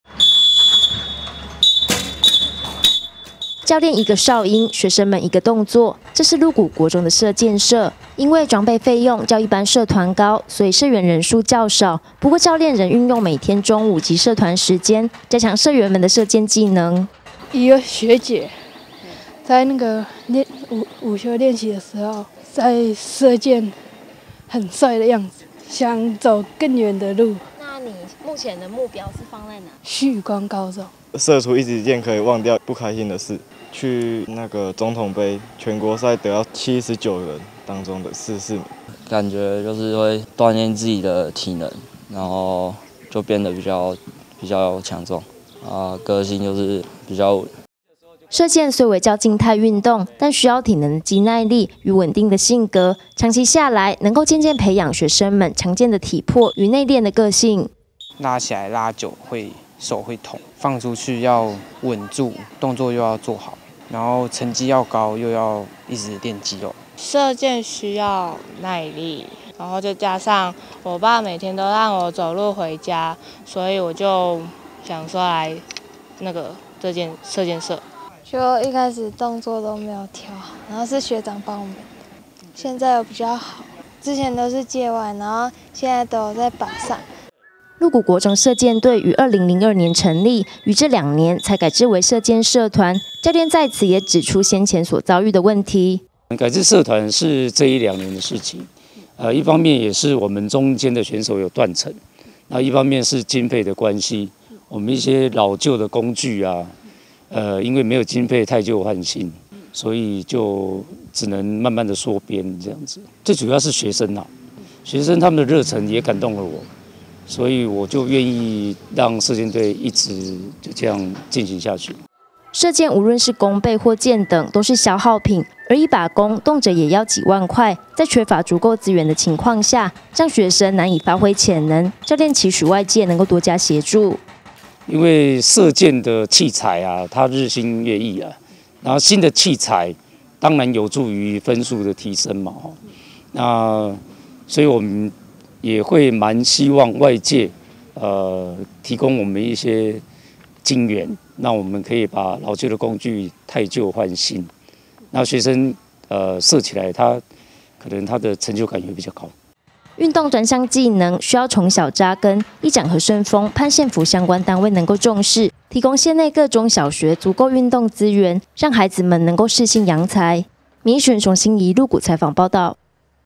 民議新聞 1040420 鹿中射箭社 內斂學生個性、強健體能
鹿谷國中射箭隊，長久以來皆有不錯成績，不過因偏鄉學校資源有限，加上前幾年人數的斷層，近兩年改制為射箭社，議長 何勝豐期盼縣府相關單位，能提供經費及資源，讓鹿中射箭社能夠培育更多優秀運動員。